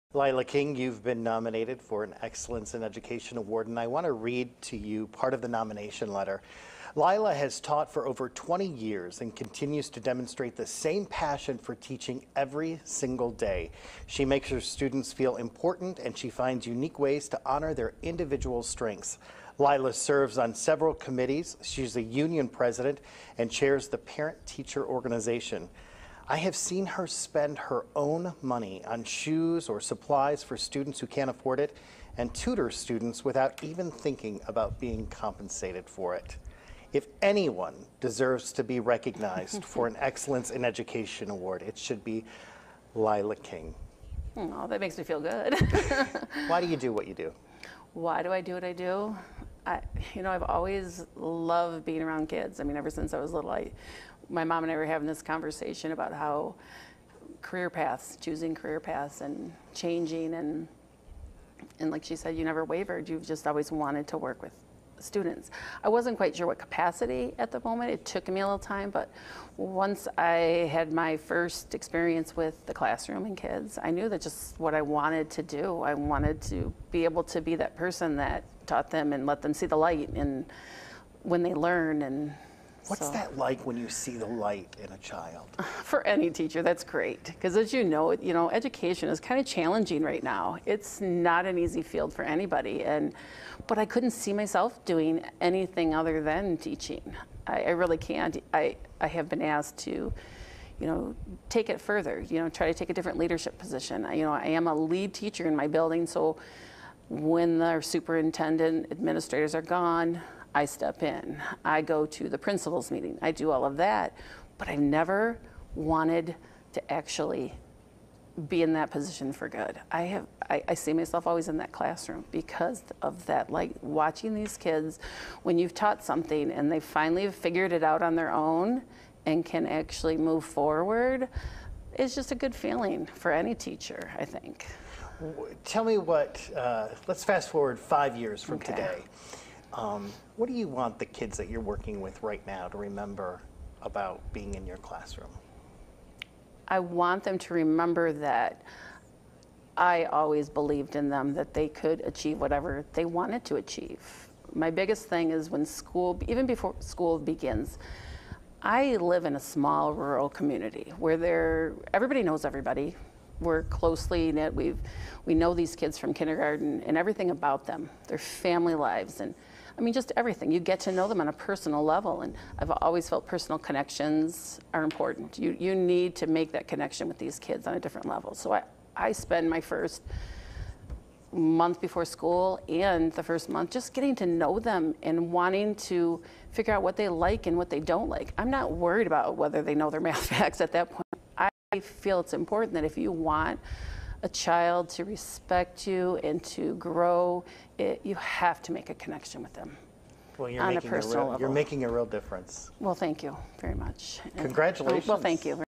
Excellence in Education Extended Cut: Lila King
A Mackinac County educator known for helping her students learn to succeed in life and the classroom through perseverance and building on their individual strengths and talents has been honored with an Excellence in Education award from the Michigan Lottery.

The award winner, Lila King, is fourth grade teacher for the Engadine Consolidated Schools in Mackinac County.  She also founded and chairs the local Parent-Teacher Organization and serves on several school committees in the Upper Peninsula district.